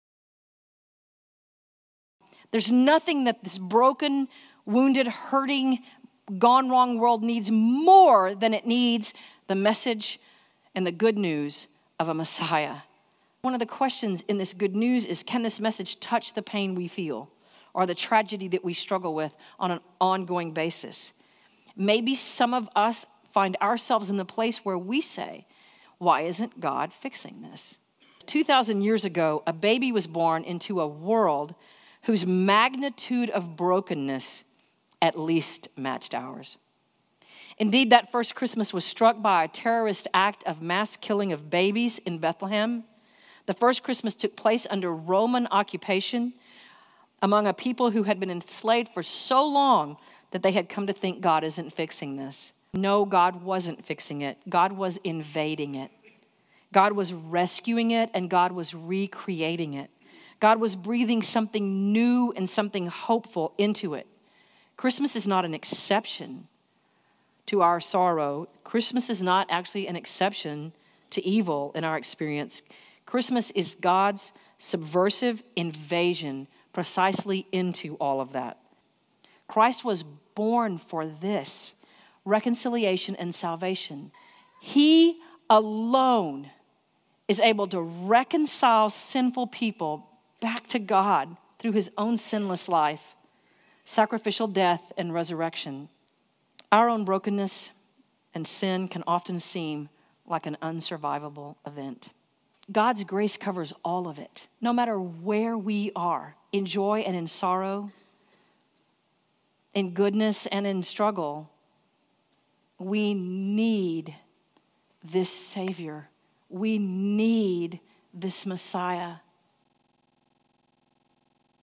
The U: University United Methodist Church Contemporary Worship Sermon Excerpt
“The Birth, the life, the death of Jesus Christ, Jesus the Messiah is a message to which no other message in all of life can ever compare.” Take two minutes with Pastor Laurinda Kwiatkowski as she uses Luke 2:8-14 in her sermon "The Messiah Song."

Watch us stream worship live to your device Sundays @ 11:00 am.